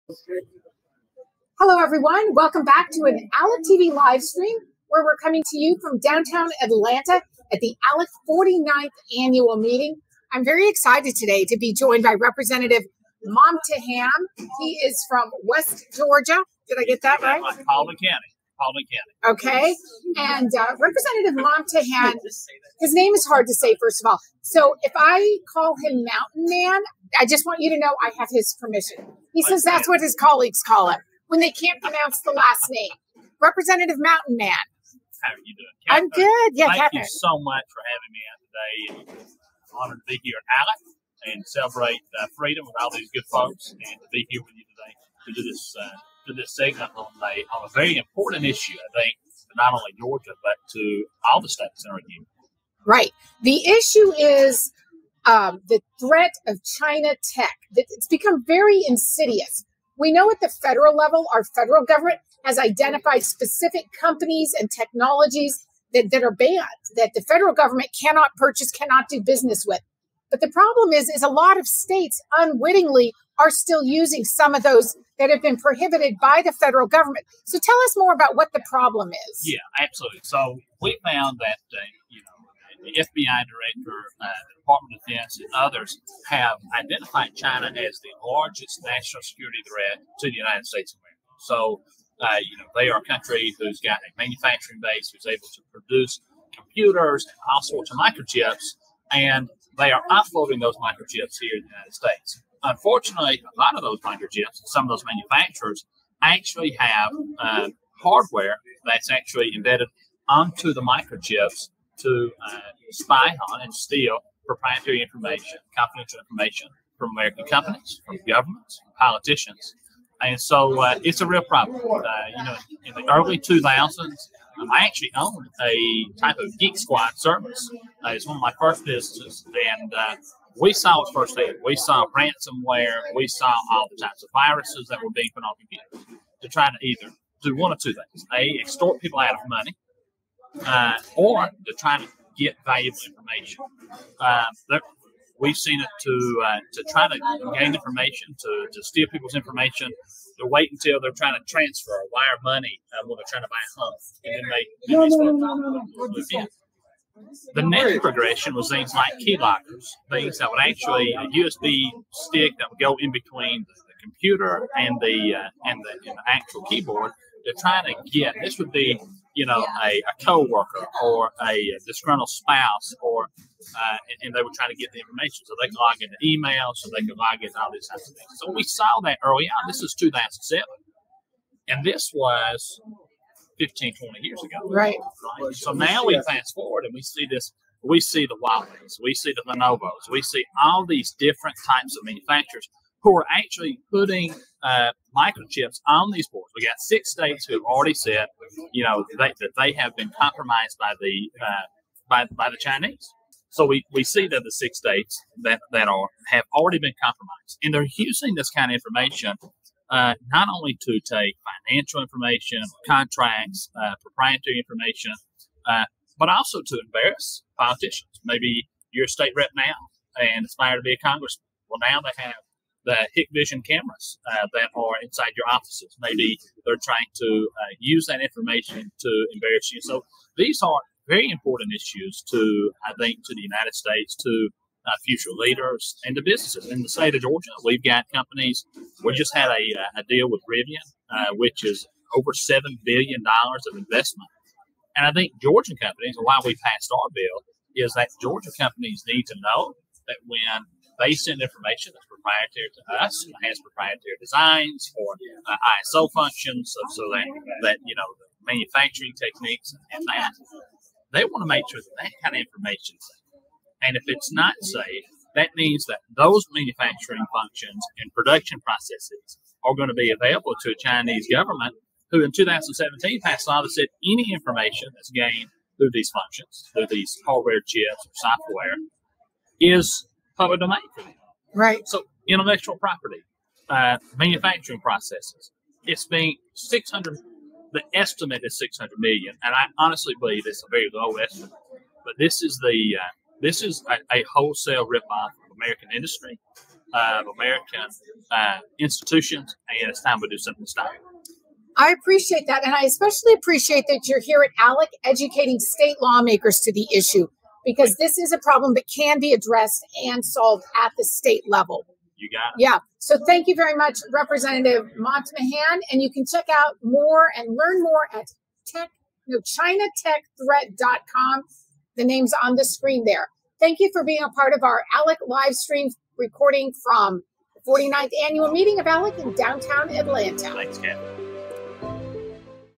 How States Can Protect Themselves from the Chinese Tech Threat
Georgia State Rep. Martin Momtahan joins the ALEC 49th Annual Meeting to educate other state lawmakers about China’s strategy to infiltrate state governments through the purchase of risky technology embedded in state level networks. Conference members considered and approved a model policy introduced by Rep. Momtahan to help states restrict the purchase of risky technology in Georgia. The model is based on a bill Rep. Momtahan led in Georgia and Governor Brian Kemp signed into law in May 2022.
ALEC adopted a model policy aimed at protecting states from the China tech threat at its 49th Annual Meeting in Atlanta